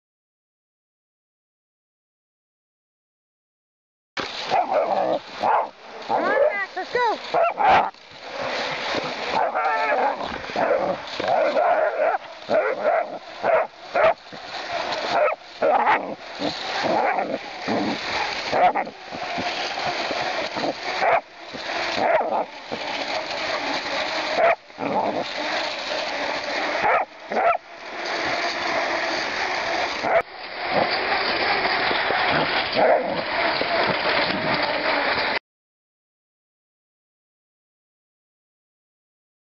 Baby Max Skis.wmv
Madicus Maximus was with a few screws loose and a real love for running and snow!  Yep, he is a Labrador, but he has the heart of a Sled Dog!